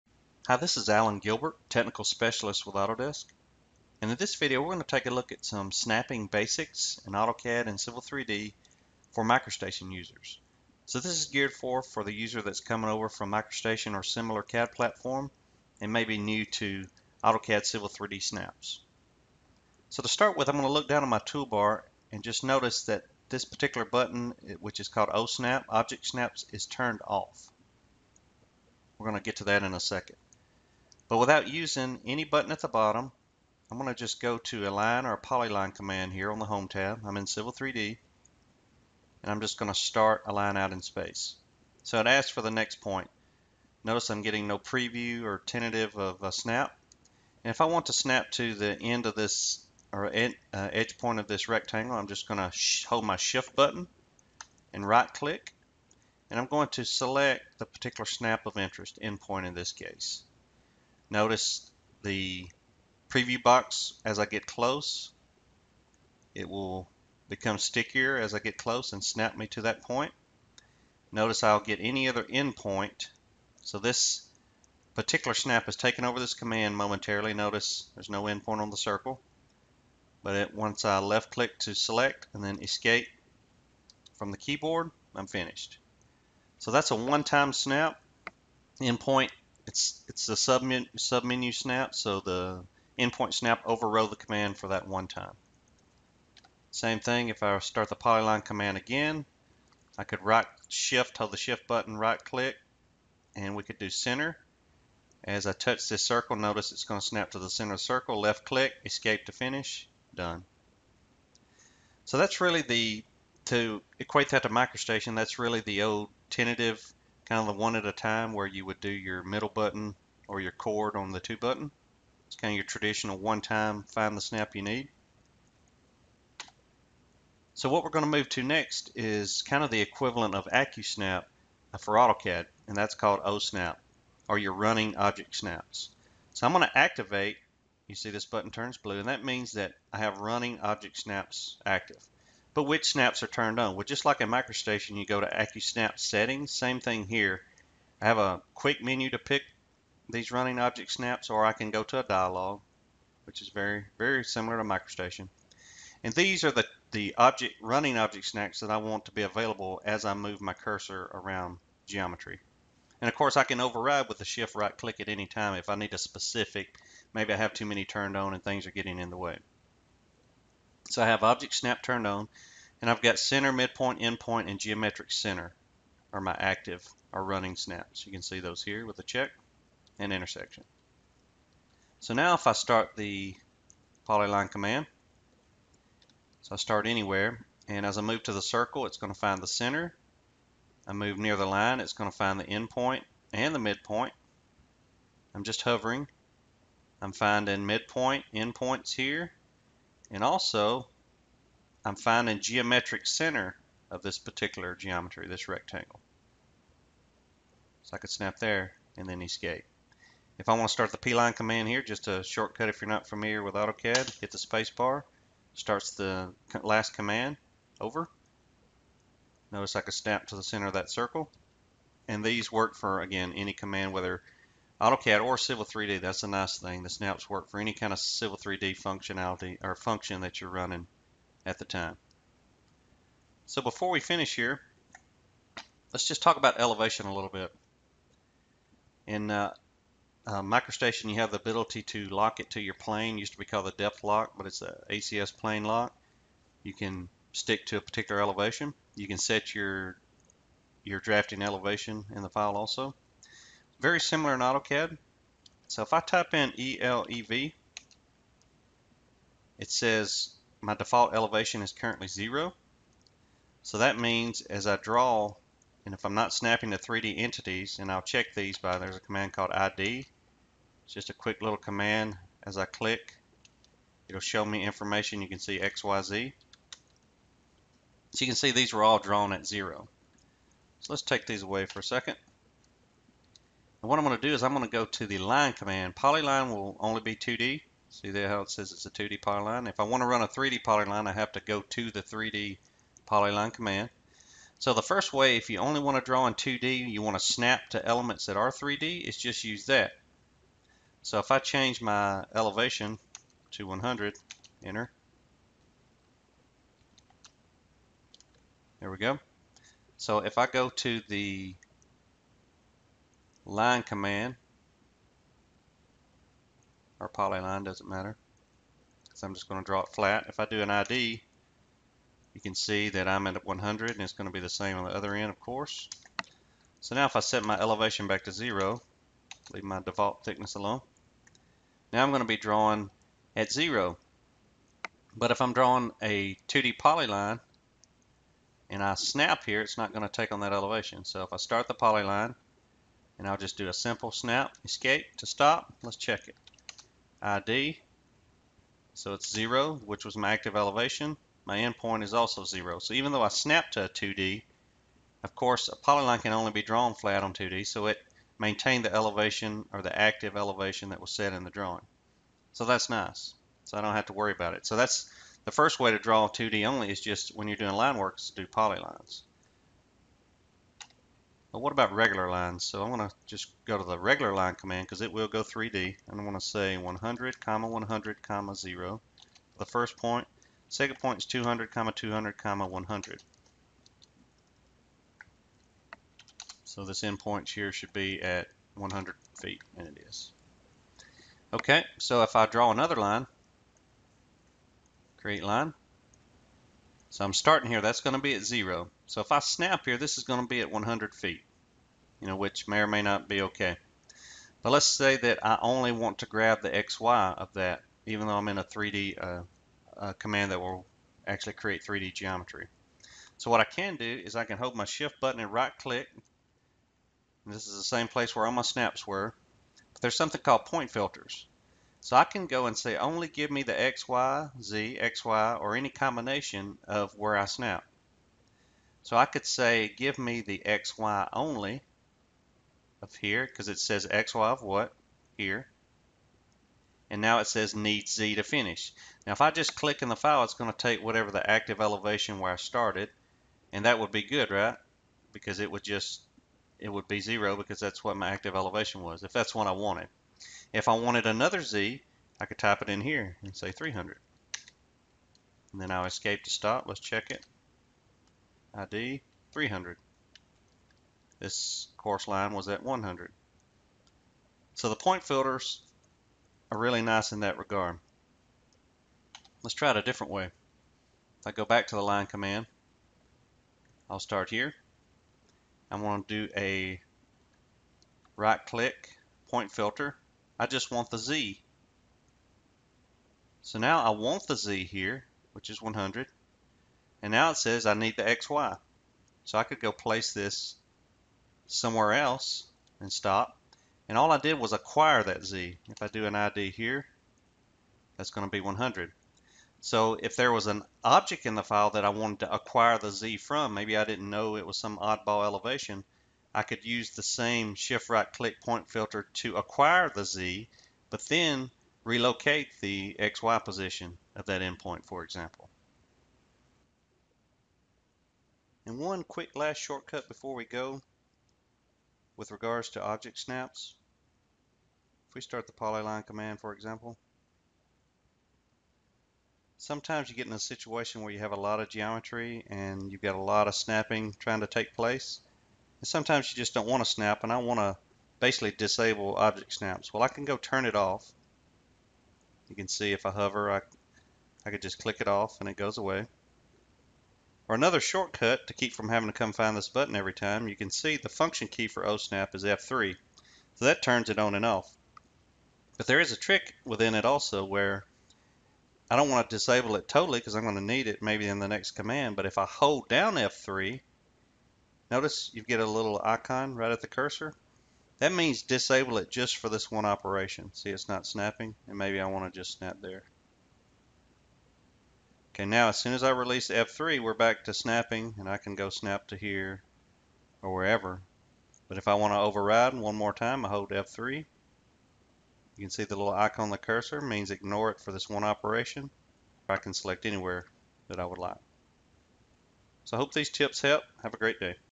Civil 3D/AutoCAD Drafting Basics for Microstation Users Pt. 2 - Snaps
In this session, we will walk through some basic snap options available in AutoCAD and Civil 3D. The primary focus of this series is to help a user with a Microstation background get up to speed with AutoCAD and Civil 3D faster.